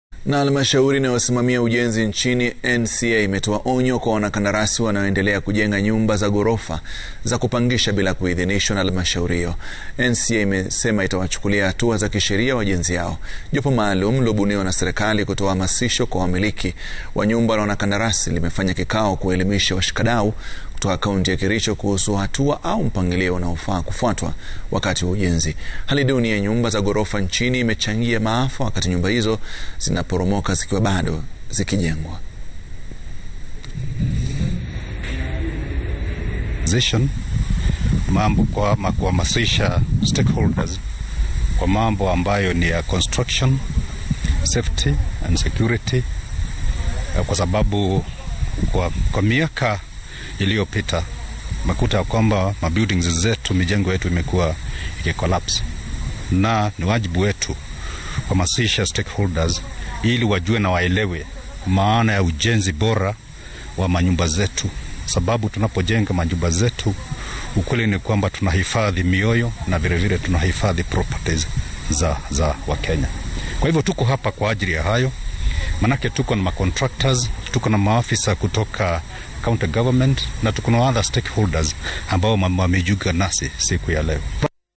Halmashauri ya ujenzi wa nyumba, NCA, yaonya wajenzi wa kiholela
Halmashauri inayosimamia ujenzi nchini NCA imetoa onyo kwa wanakandarasi wanaoendelea kujenga nyumbza za gorofa za kupangisha bila kuidhinishwa na halmashauri hiyo.
NCA imezema itawachukulia hatua za kisheria wajenzi hao. Jopo maalum iliyobuniwa na serekali kutoa hamasisho kwa wamiliki wa nyumba na wanakandarasi limefanya kikao kuwaelimisha washikadau kutoka kaunti ya kericho kuhusu hatua au mpangilio unaofaa kufuatwa wakati wa ujenzi . Hali duni ya nyumba za gorofa nchini imechangia maafa wakati nyumba hizo zinapoporomoka zikiwa bado zinajengwa.